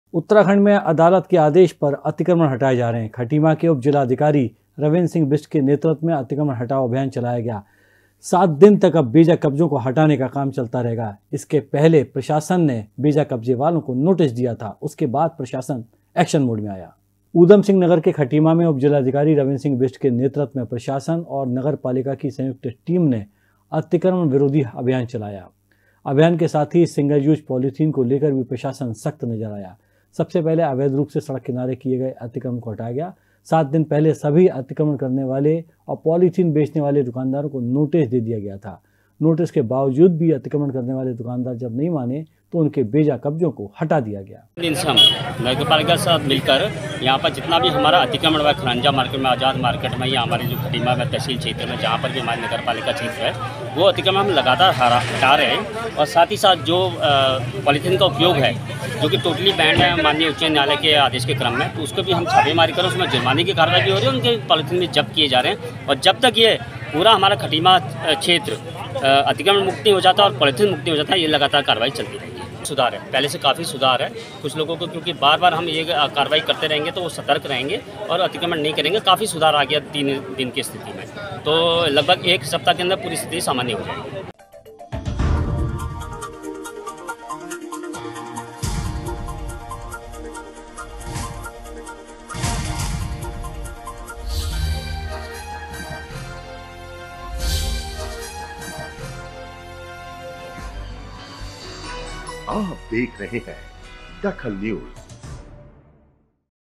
DAKHAL NEWS खटीमा में चलाया गया अतिक्रमण हटाओ अभियान
खटीमा में चलाया गया अतिक्रमण हटाओ अभियान
7  दिनों  तक हटाए जाएंगे लोगों के बेजा कब्जे